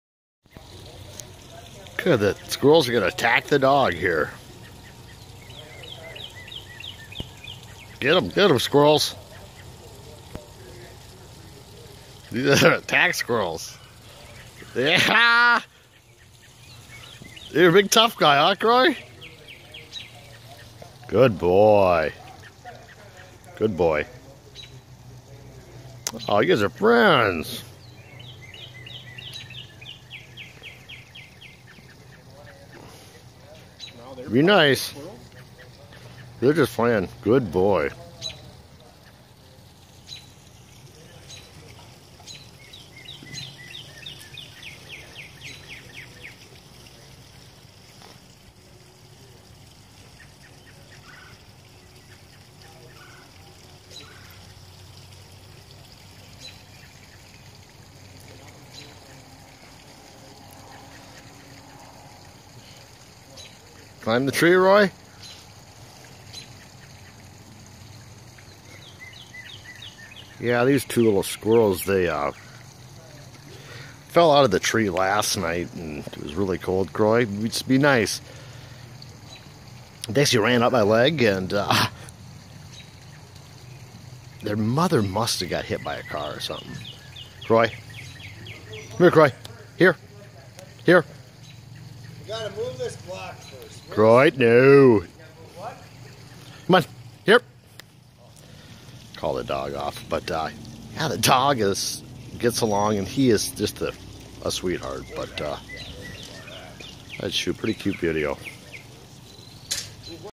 Unusual Animal Friends-Dog and Baby Squirrels
We had three baby squirrels come out of their nest this spring while we were working in the yard. The mother was nowhere to be found. These baby squirrels started playing with our dog.

I grabbed my phone and started recording because I could not believe it. They played for several hours until it got dark. I have not seen these two since but I hope they are doing well.